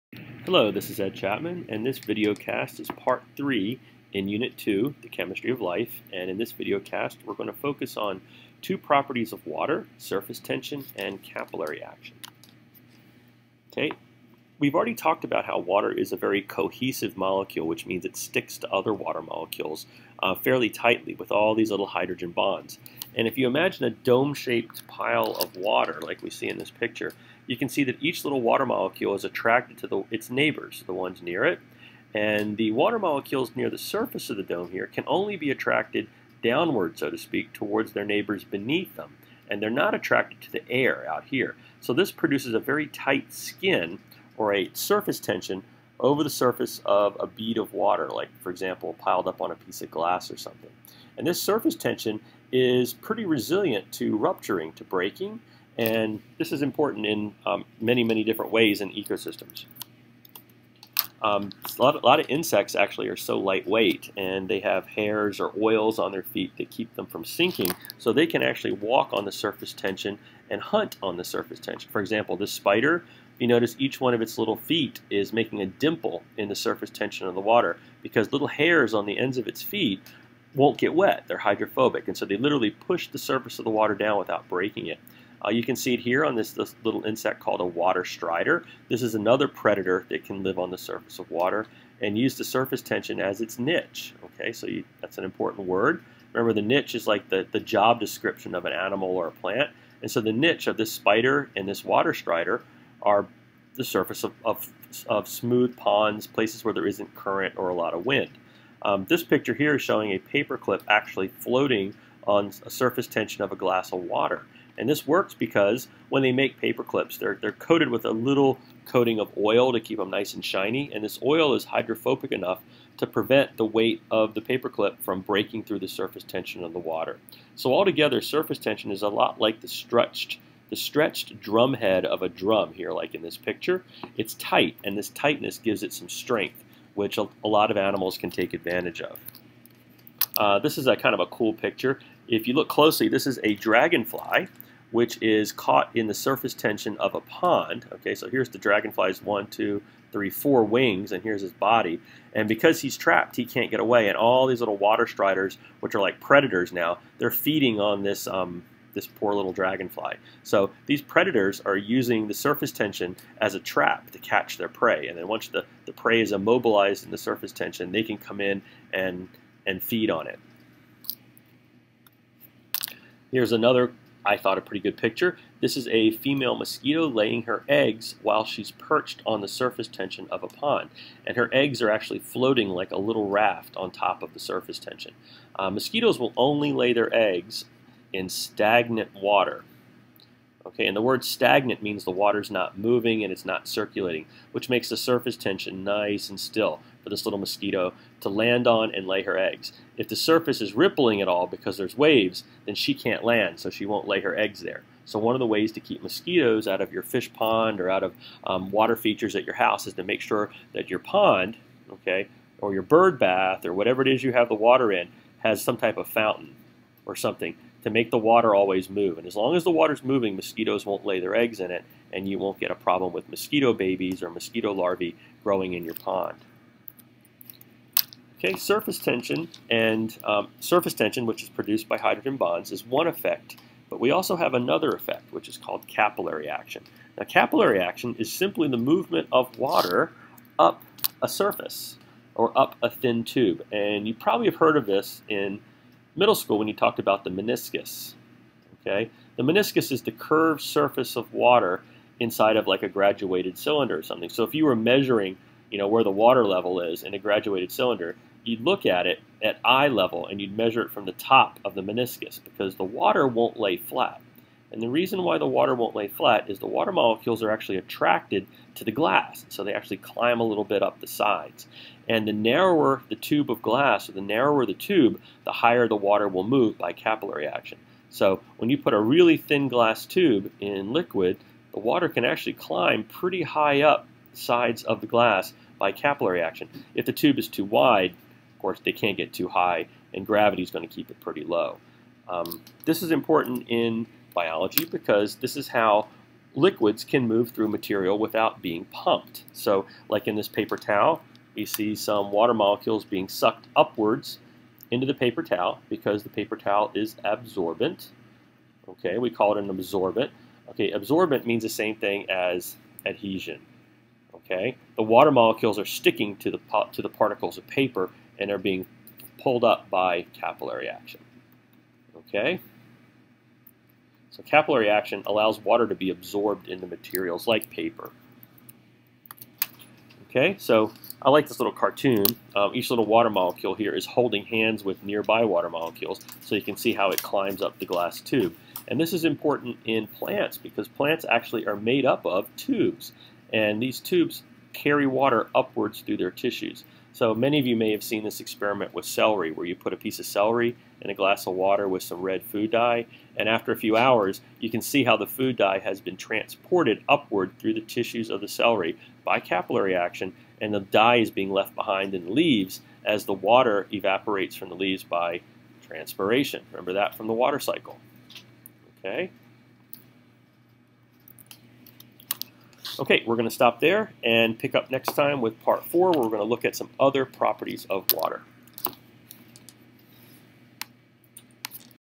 Unit 2 Pt 3 Surface Tension & Capillary Action
Surface tension & Capillary Action